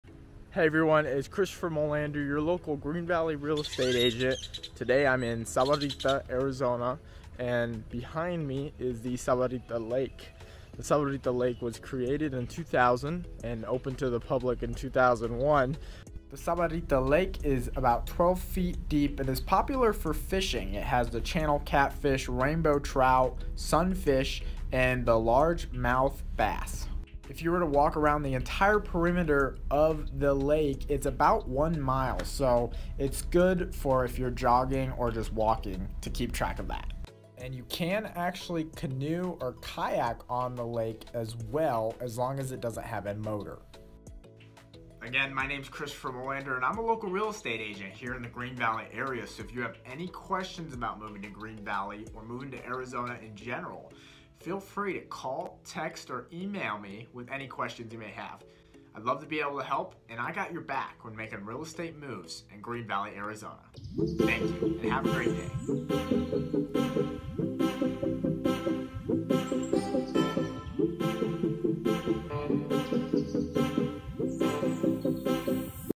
Fast Intro to Sahuarita Lake | Rancho Sahuarita | Tuscon Arizona
Welcome to Rancho Sahuarita, a beautiful community nestled in Southern Tucson, Arizona. In this fast-paced introduction video, we invite you to explore Sahuarita Lake, one of the highlights of this thriving neighborhood.  Watch this fast-paced introduction video now and start planning your visit to this idyllic oasis in the desert.

Christopher Molander- Call, Text, or Email anytime!